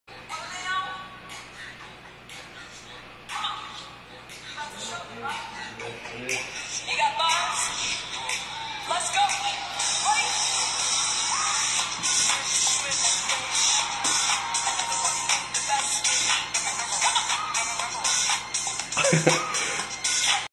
Davi dançando floss dance
Meu sobrinho dançando floss dance hehe